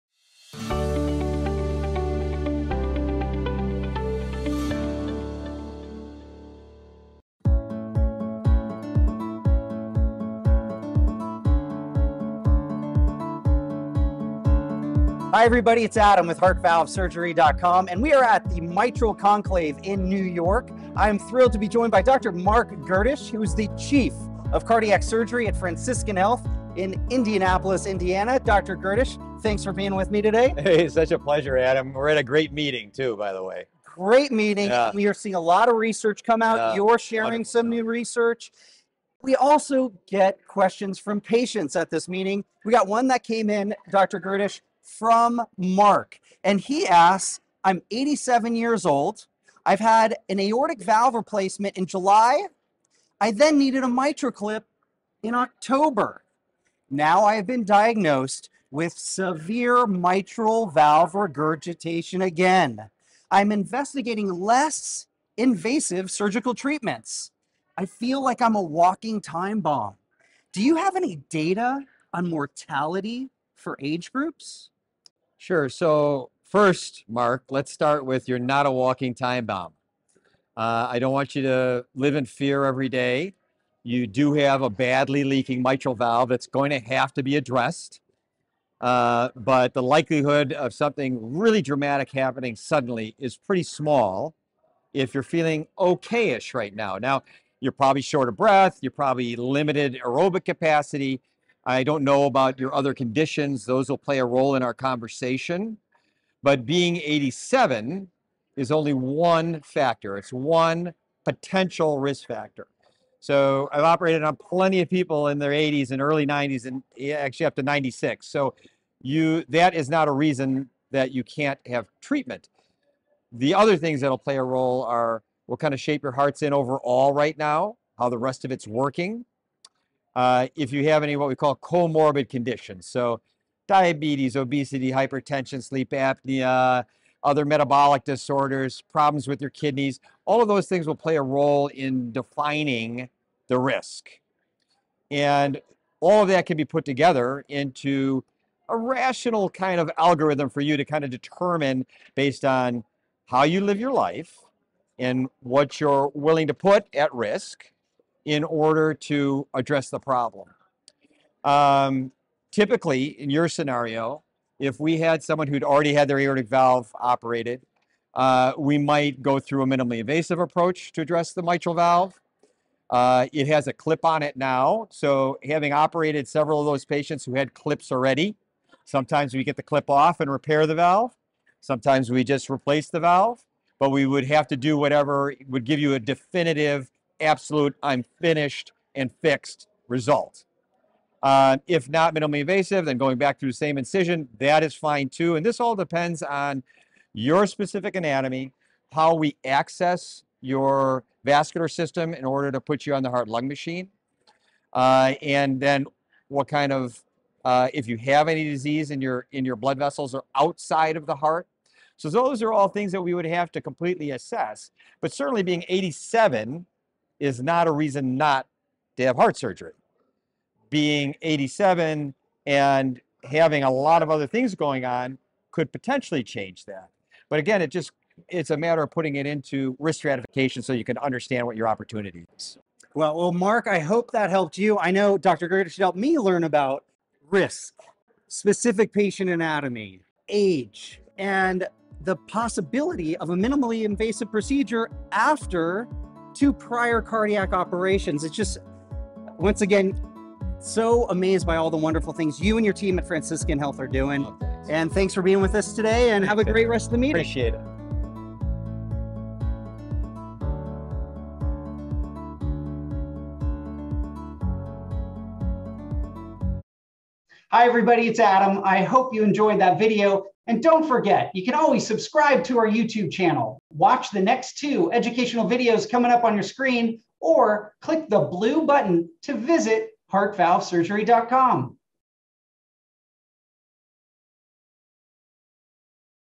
Risk, Mortality & Age: What Should Heart Valve Patients Know?
- Patients diagnosed with severe heart valve disease are often confused and have questions about their surgical risk, mortality rates and the potential impact of age on outcomes.

For example, Mark asked us, "I am 87 years old. I’ve had an aortic valve replacement in July. I then needed a MitraClip in October. Now, I have been diagnosed with severe mitral valve regurgitation again. I am investigating less-invasive surgical treatments. I feel like I’m a walking time bomb. Do you have any data on mortality for age groups?"

To answer that question, we interviewed Dr. Marc Gerdisch, Chief of Cardiac Surgery at Franciscan Health in Indianapolis, Indiana.  Dr. Gerdisch is a heart valve expert having performed over 4,000 heart valve repair and replacement operations.